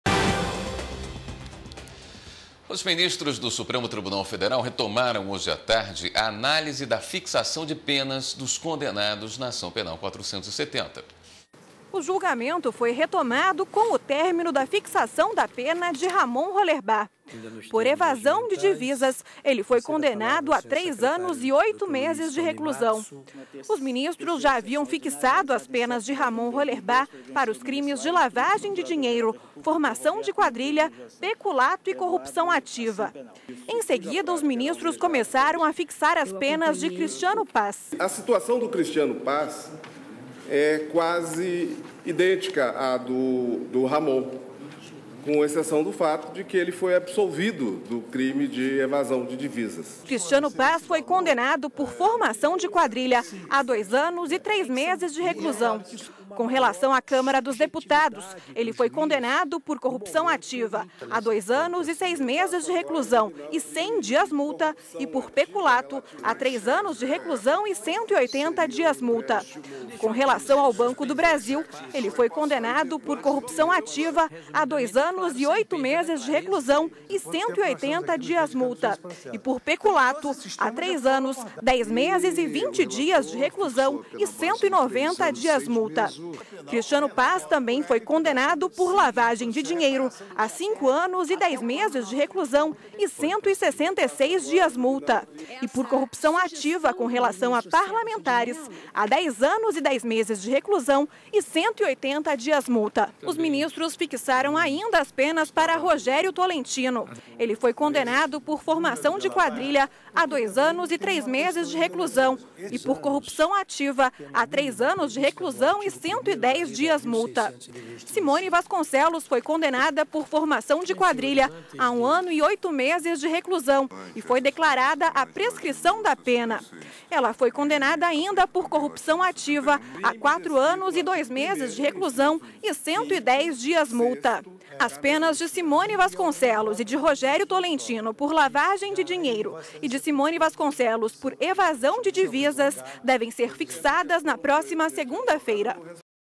Fixação de penas (08/11/12)
Os ministros do Supremo Tribunal Federal (STF) retomaram a análise da fixação de penas dos condenados na Ação Penal 470.